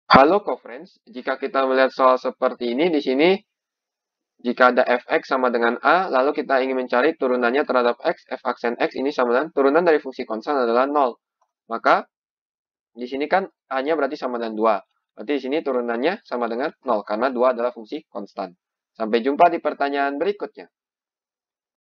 Tentukanlah turunan fungsi-fungsi berikut. f(x)=2
Tentukanlah turunan fungsi-fungsi berikut.

f(x)=2

Topik atau Materi: Turunan Sebagai Limit Fungsi - Turunan - KALKULUS - Matematika Kelas 11 SMA - K-13

Sumber Soal: Matematika Kelas 11 Wajib, K-13, MK, 2015 , SEWU

00:00 : Bedah Soal
01:00 : Konsep, rumus dan pengertian Turunan Sebagai Limit Fungsi
01:18 : Langkah penyelesaian soal
-1:21 : Jawaban akhir
-1:51 : Penutup